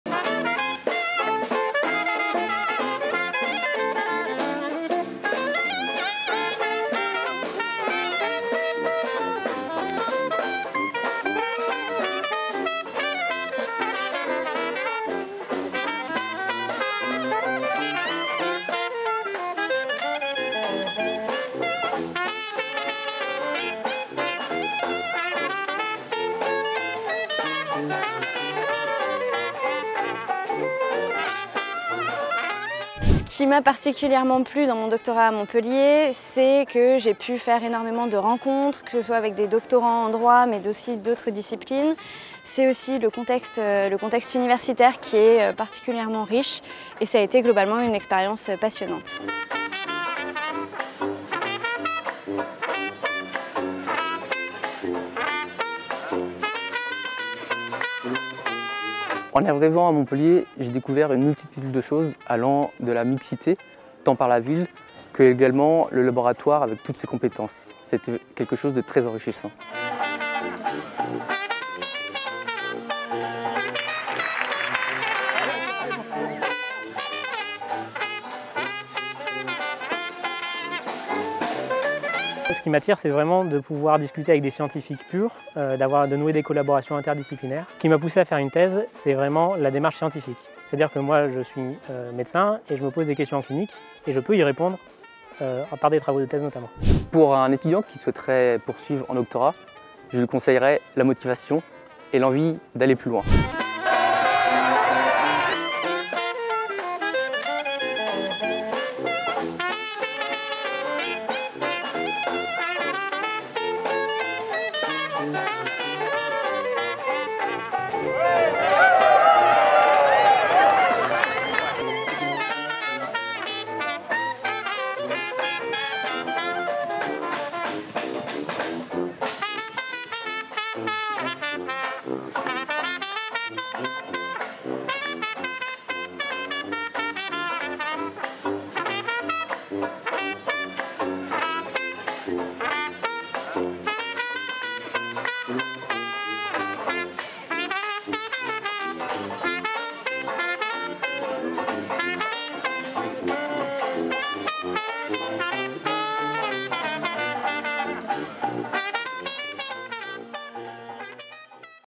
Remise des diplômes de doctorat - Université de Montpellier Promo 2021
Retour en images de la Cérémonie de remise des diplômes de doctorat, promotion 2021 du Collège Doctoral de l'Université de Montpellier. A l'Opéra Berlioz - Corum Montpellier et au Jardin des Plantes